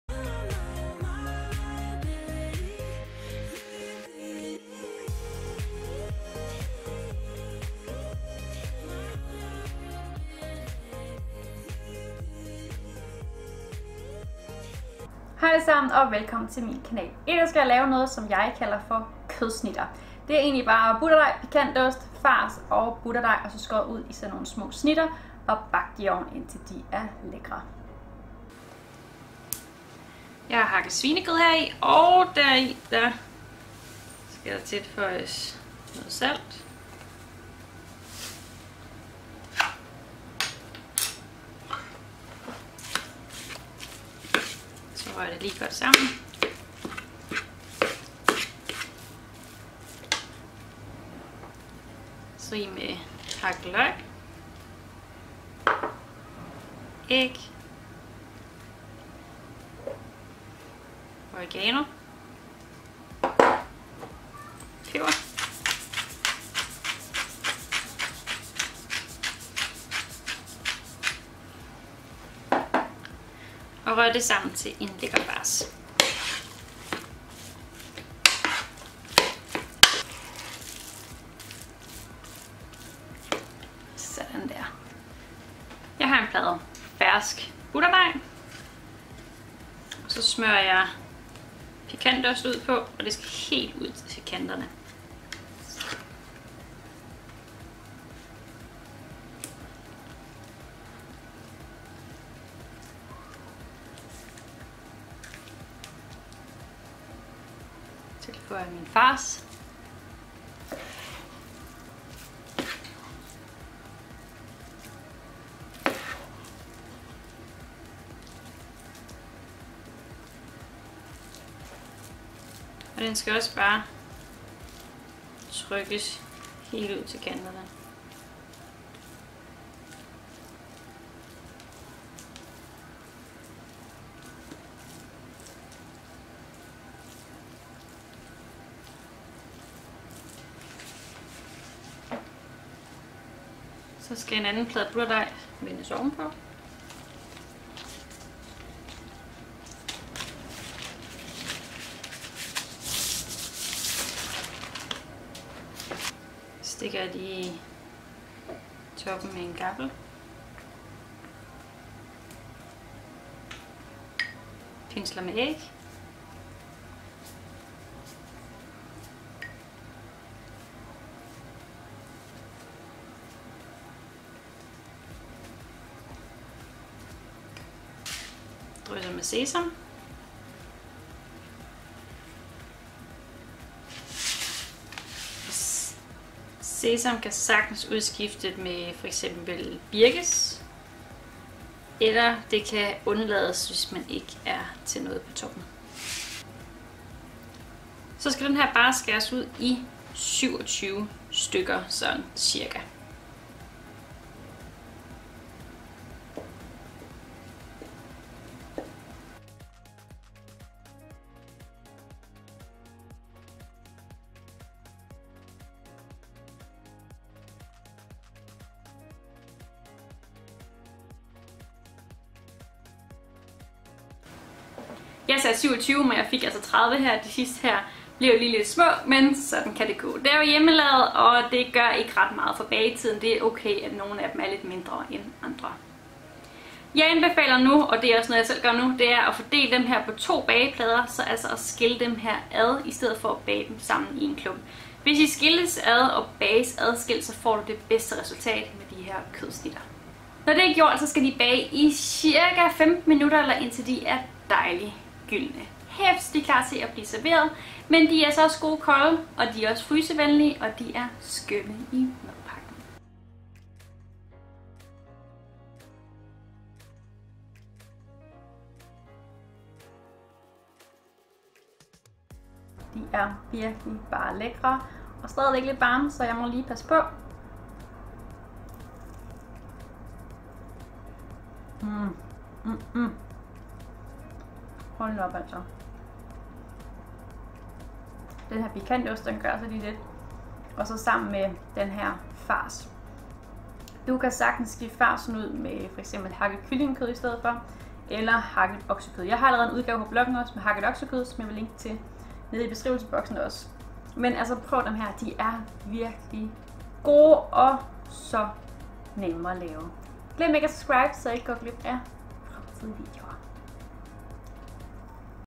Kødsnitter - Butterdejshapsere Med Fars Og Pikantost
Lækre kødsnitter! Navnet er måske lidt mærkeligt, men jeg blev inspireret af "snitten", da jeg kom på denne opskrift. Butterdej, pikantost, fars og butterdej igen og så skåret ud i snitter. Herligt i madpakken og skøn snack. De kan nydes både varme, kolde og de kan fryses.

Mit ene kamera som filmer mig og ikke maden, var ikke tændt, da jeg lavede maden (en fejl, desværre). Så lyden er lidt anderledes, da mikrofonen er tilkoblet det kamera, som filmer mig. Men jeg håber det går! :-)

27-30 stk
Du skal bruge:
2 plader fersk butterdej 2×275 gram
200 g pikantost eller anden flødeost
500 g hakket svinekød
1½ tsk salt
Peber
1 løg
1 æg
1 spsk oregano

Til toppen
1 letpisket æg
Sesam

180 grader i cirka 15 minutter